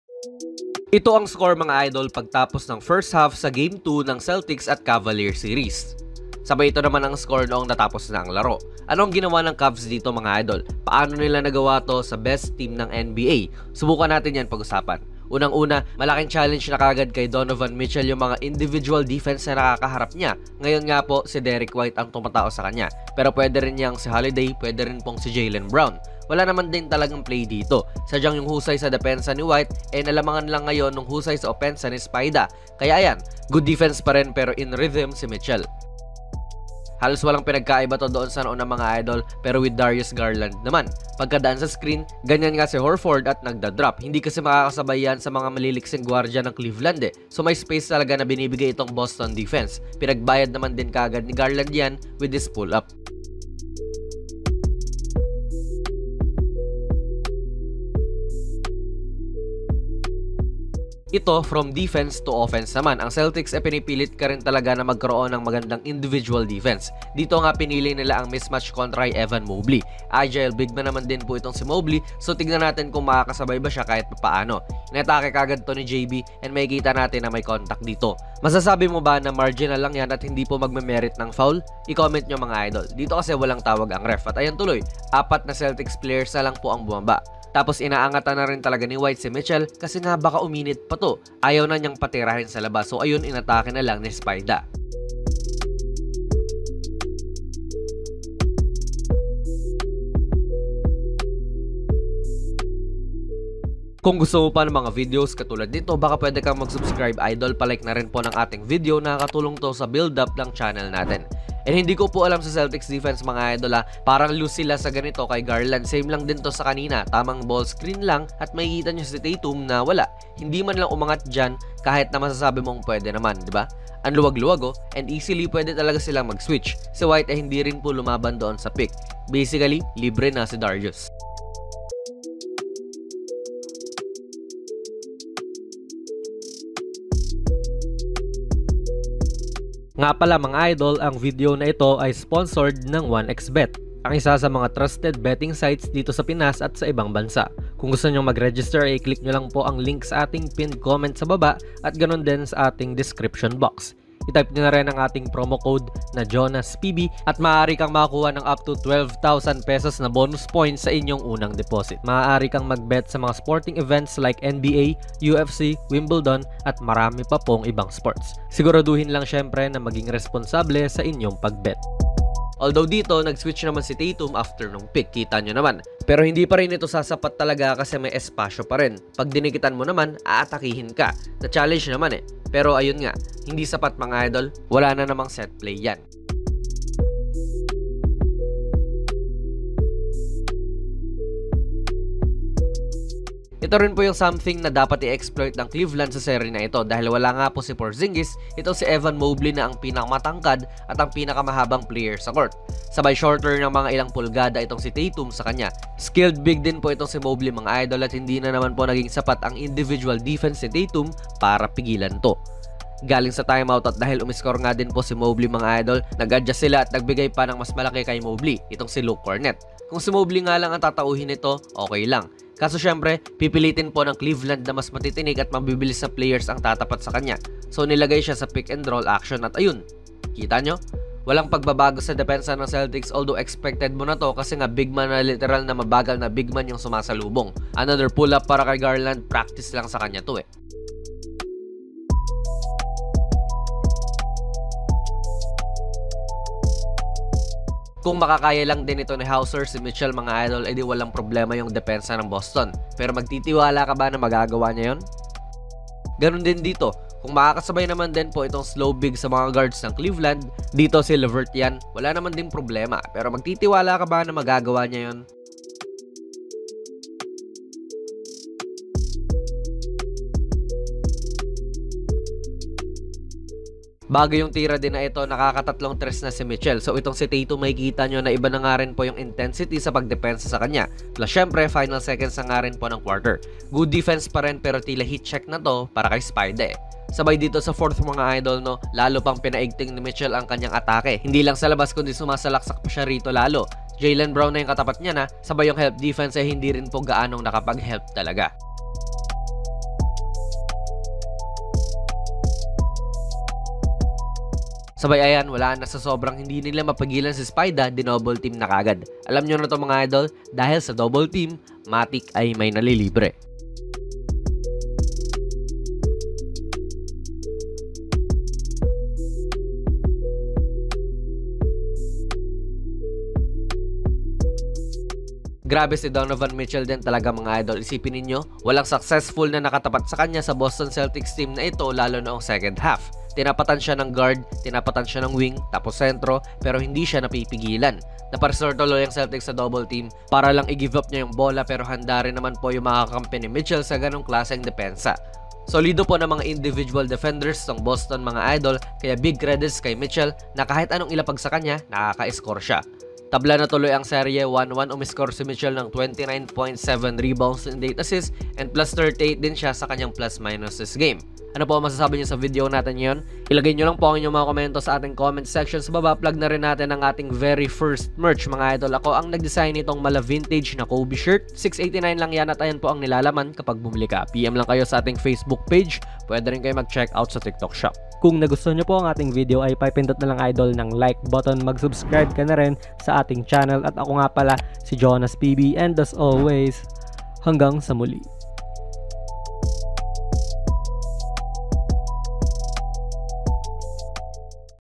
Ganito SUMABOG BIGLA ang CAVALIERS para TAMBAKAN nag Celtics sa GAME 2!
Medyo kagagawan din ng Celtics, pero lakas lang din ni Mitchell this game!